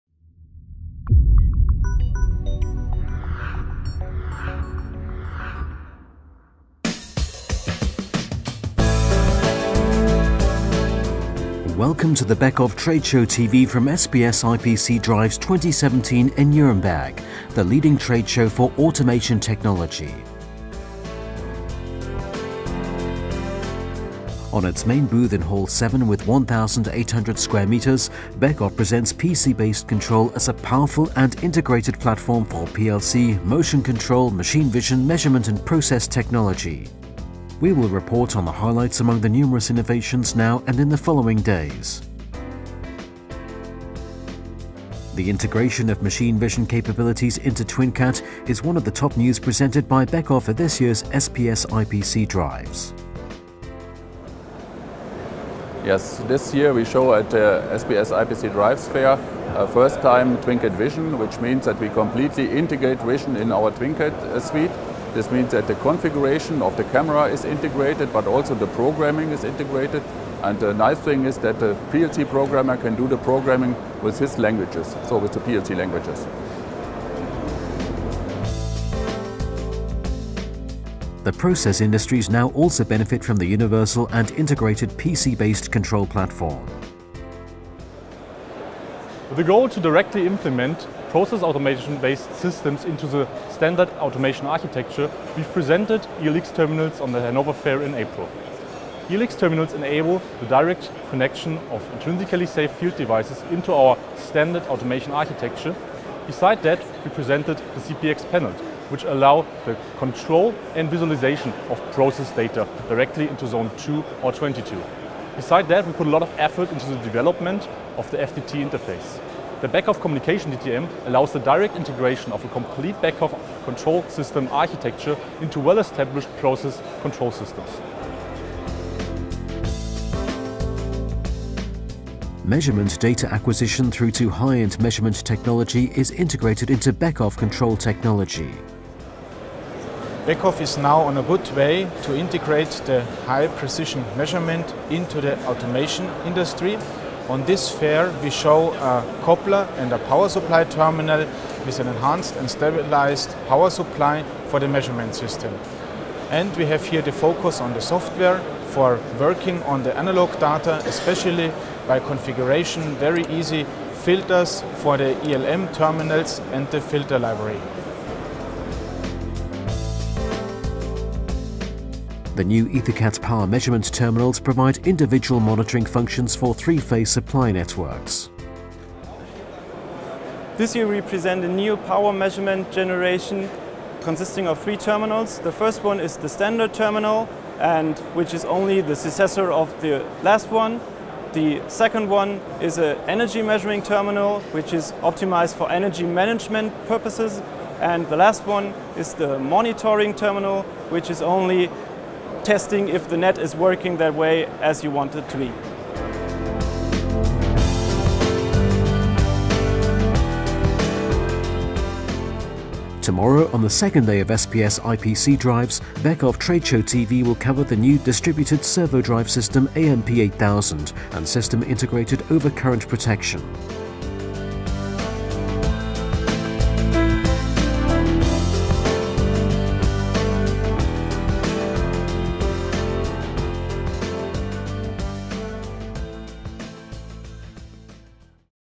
SPS IPC Drives 2017, Day 1: Beckhoff Trade Show TV
Beckhoff Trade Show TV from day 1 at SPS IPC Drives 2017. Topics: Fully system-integrated – machine vision with TwinCAT Vision, Products for process technology up to Zone 0 hazardous areas, High-end measurement technology with EtherCAT measurement modules.